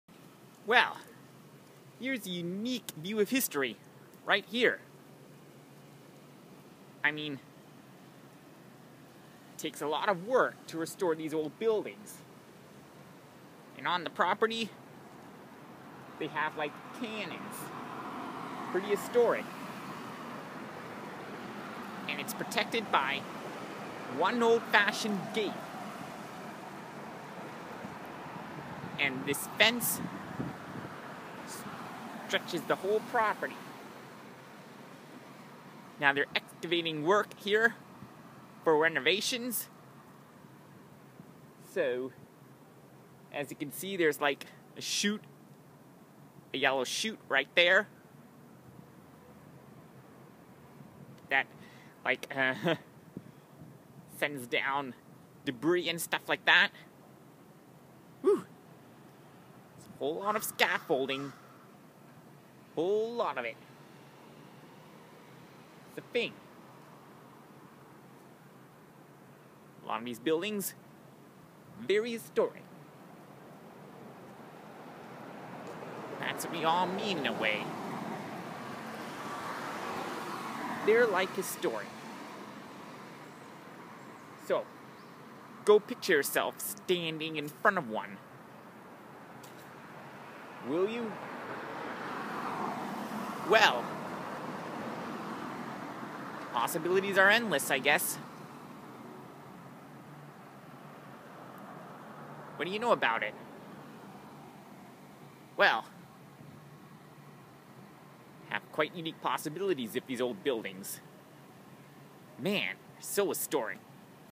Reno Building
The Nova Scotia Legislature under renovations with barriers all around the building.